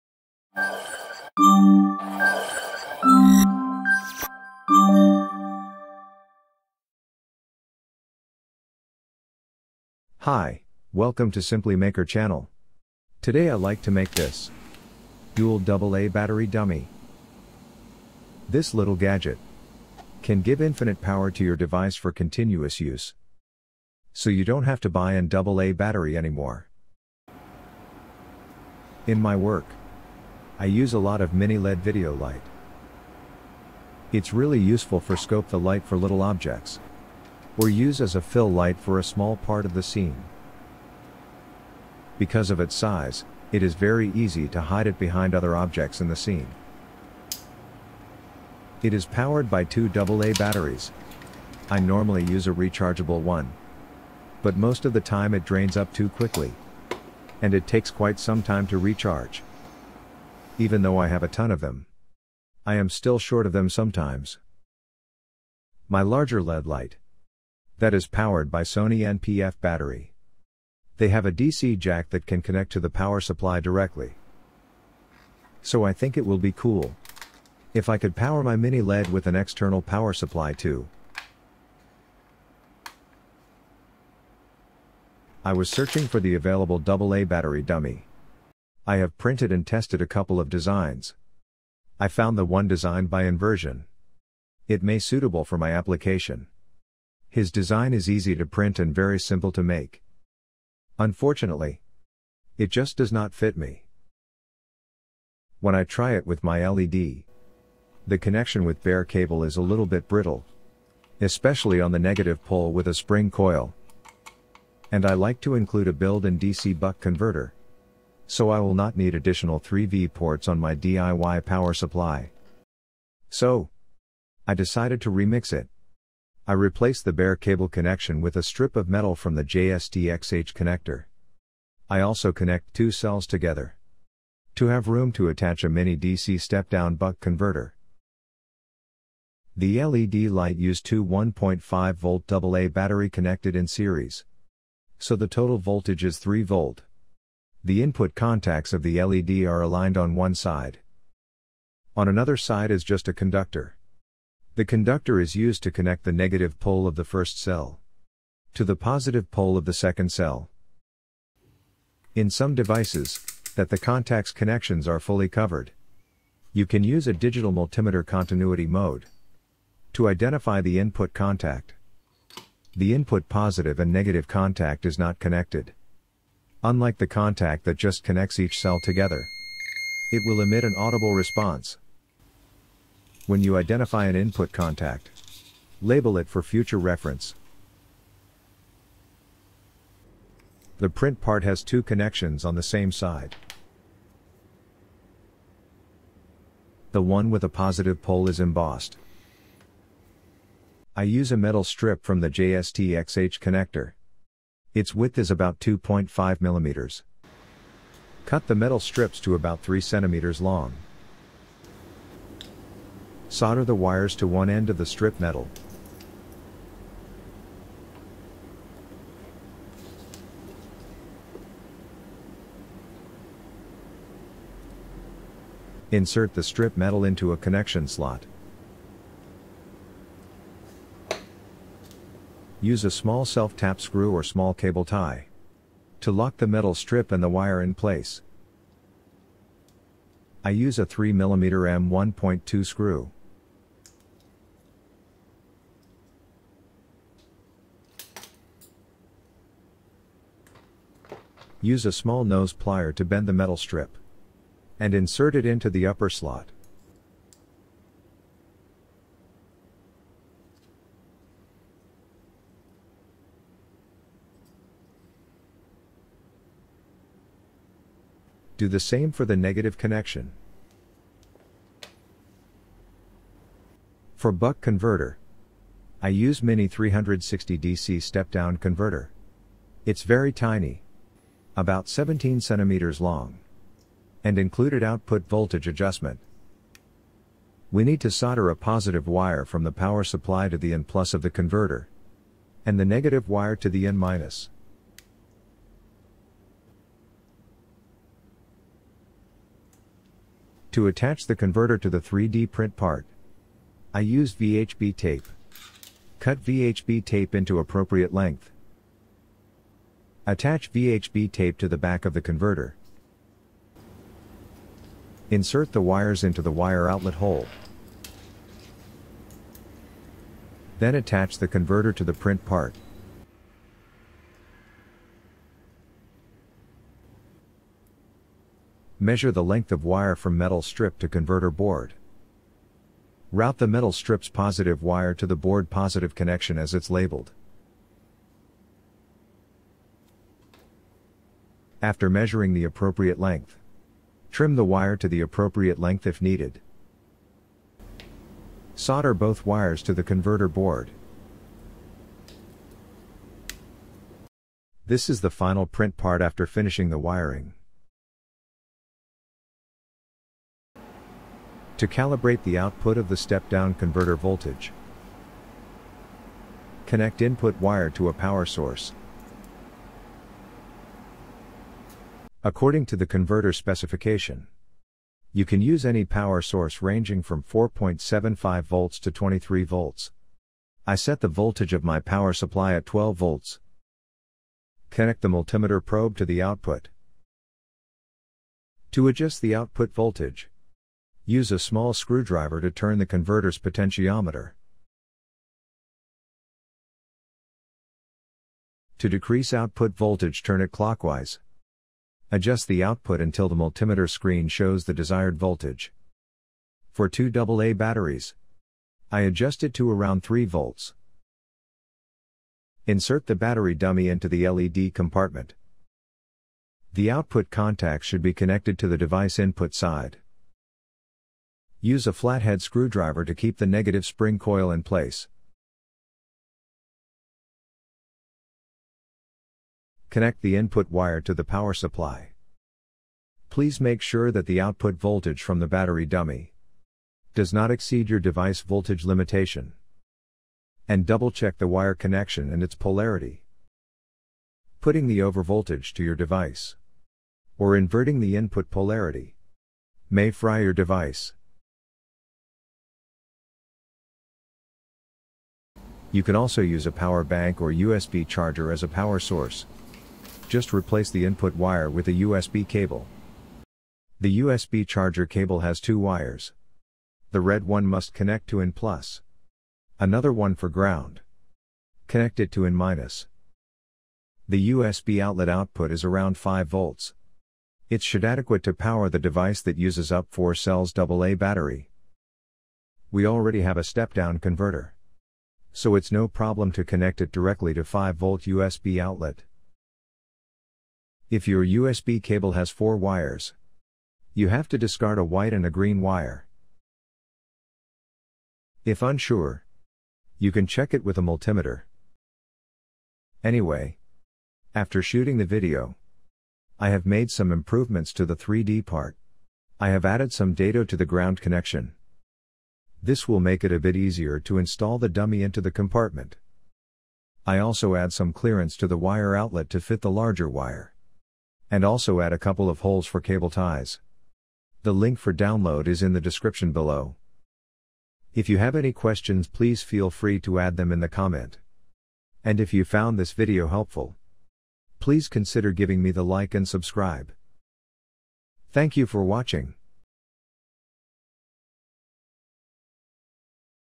Infinite power for an AA battery devices
Easy to make 3d print dual AA battery dummy with a build-in buck converter.

This battery dummy can power any devices that use an AA battery from any power source. It accepts input ranges from 4.5v to 23v. You can also use a power bank or USB charger as an input power supply.

In the video, I show you the dual cell version used with a mini LED video light. The single cell version is also available to download.

00:00 The Design
02:19 AA Battery Basic
03:10 The Make
06:00 Step-down converter calibration
07:35 Use with power back or USB outlet

► COMPONENTS FOR THIS PROJECT
● MINI-360 Buck Converter
● Selftap Mini Screws
● JST XH2.54 Connector Set
● Mini LED Light
● UNI-T UT89X Digital Multimeter

► FILAMENT USE IN THIS PROJECT
● eSun PLA+